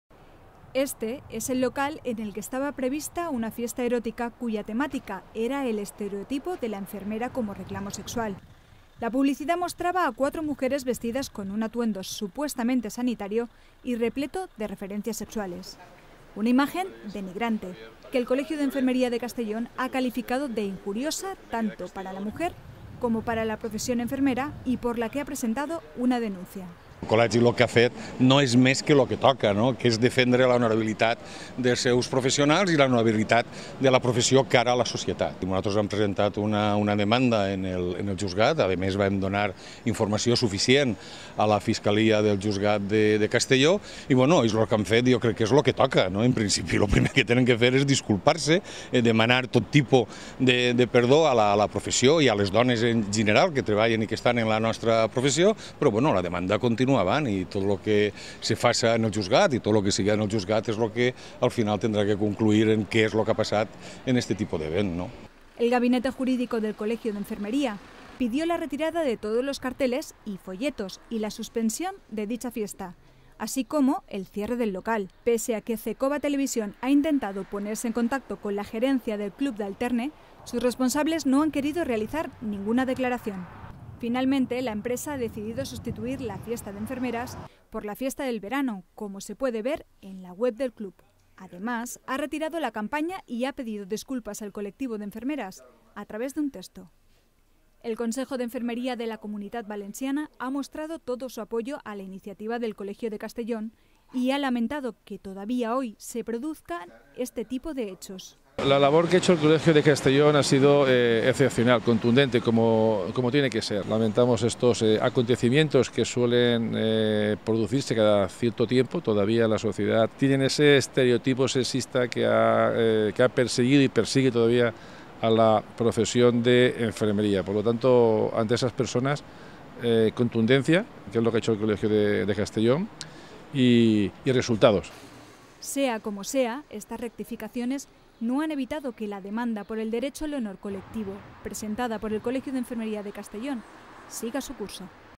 El Colegio de Castellón denuncia una fiesta injuriosa | Cecova TV
El Colegio de Enfermería de Castellón ha presentado una denuncia contra un establecimiento de alterne de Almassora por organizar una fiesta en la que se usaba la imagen de la mujer y de Enfermería como reclamo sexual.